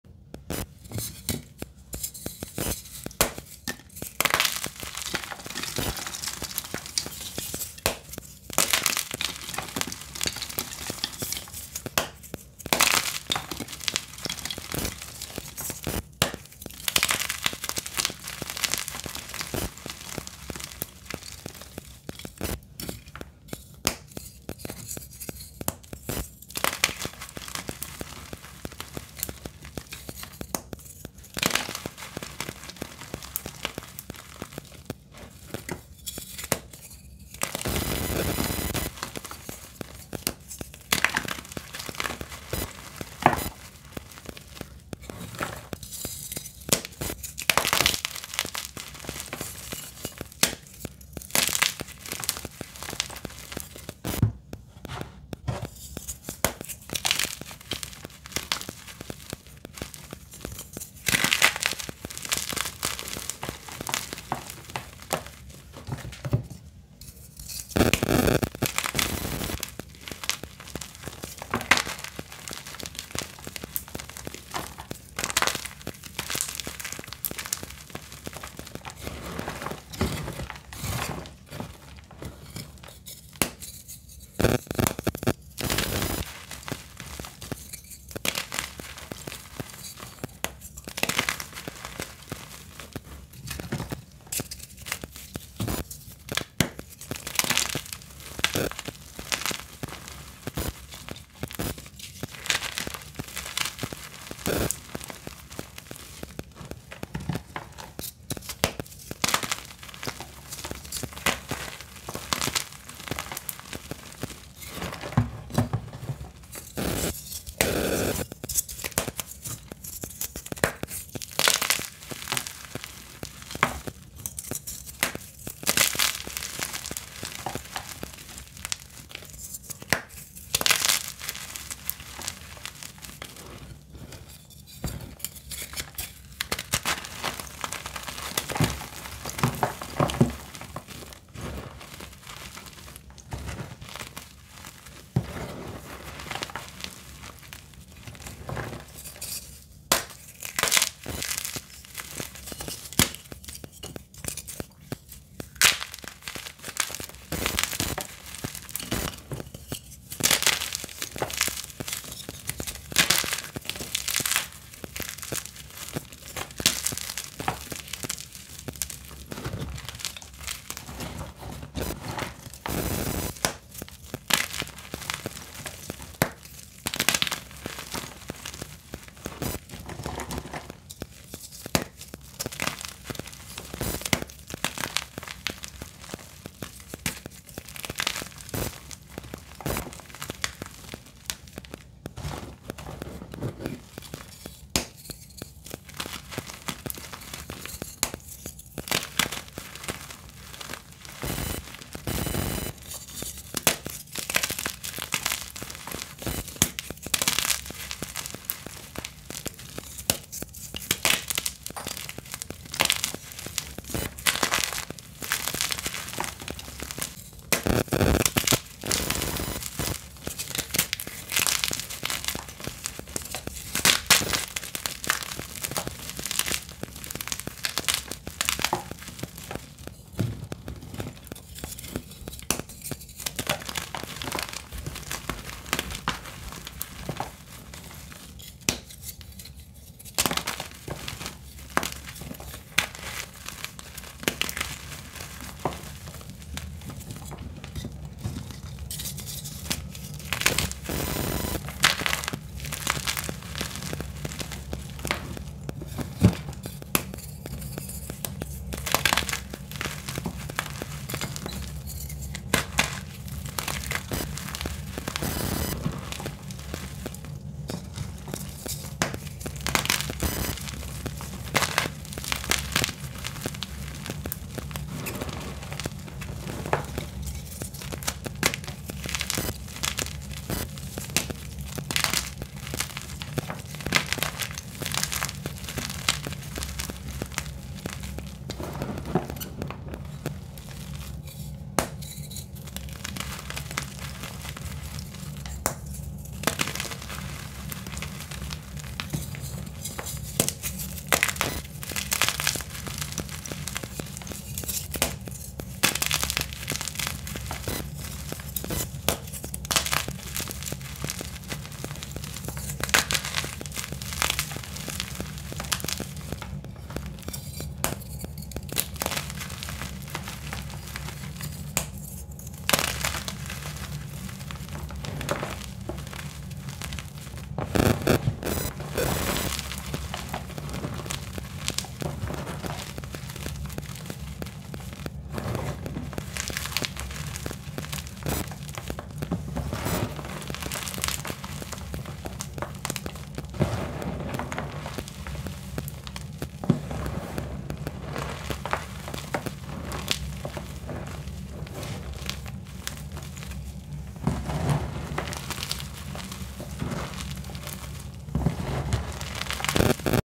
WARNING: POOR AUDIO - SUGGESTIONS NEEDED - GYM CHALK ASMR
Hello, my dear friends on this channel.

I just uploaded this video to show you what has been happening with my blue yeti mic over the past week or so. I've had this particular mic for over 3 years now, and now it's acting up.

I need suggestions on ways to fix this. These mics cost up to 200 dollars, which is quite a bit of change. Any suggestions will be welcome.

I'm looking forward to creating more once I fix up this mess.

pearly